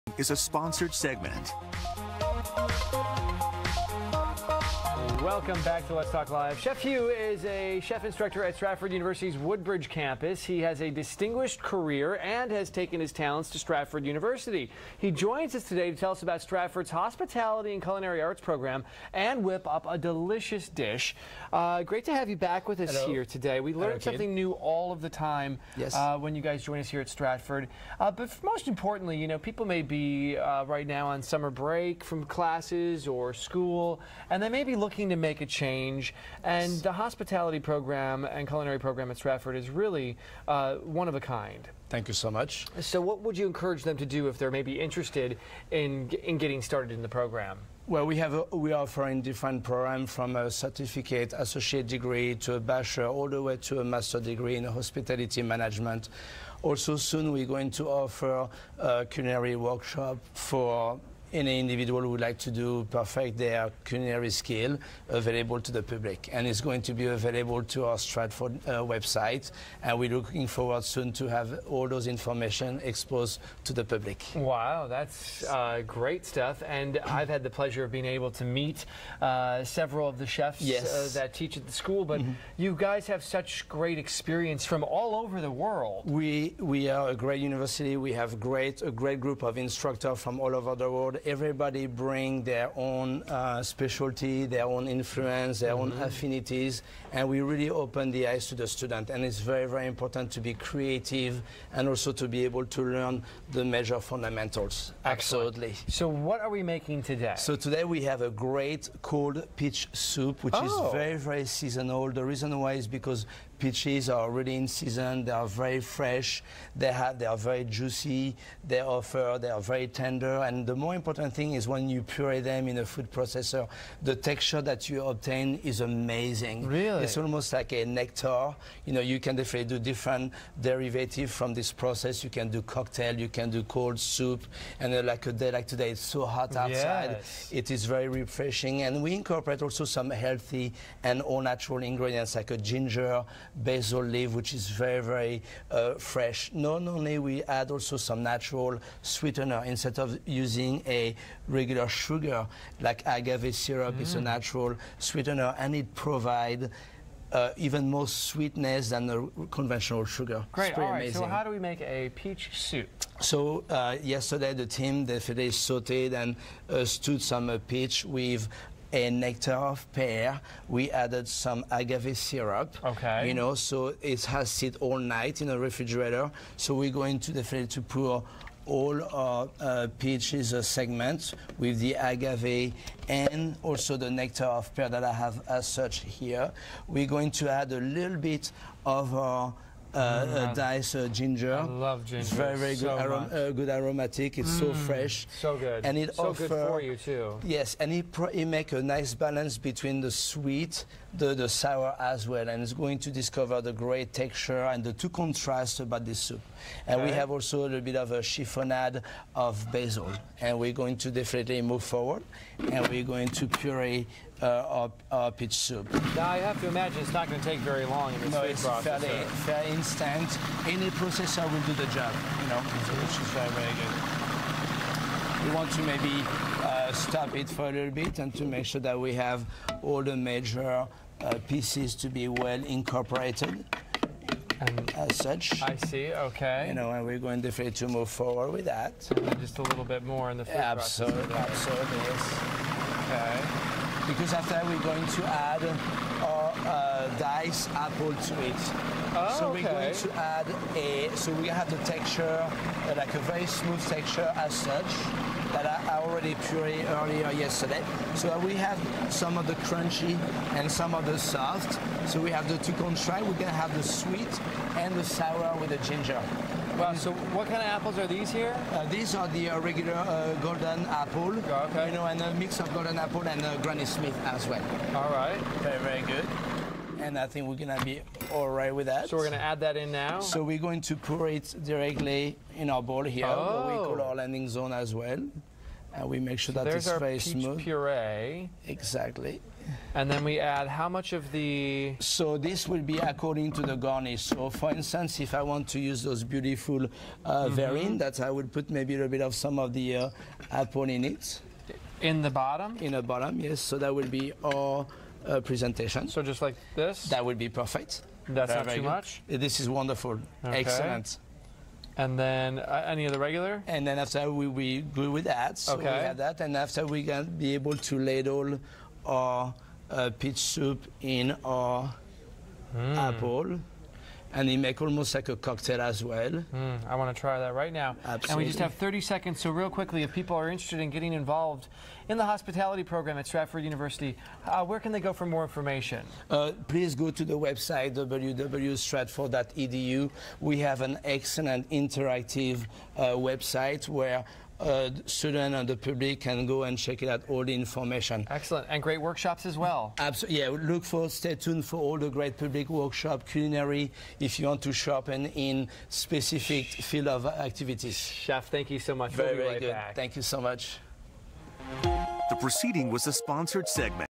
Chef Coussard Cold Peach Soup 7 12 17
Cooking Demo with Chef Cossard - Stratford University - Woodbridge Campus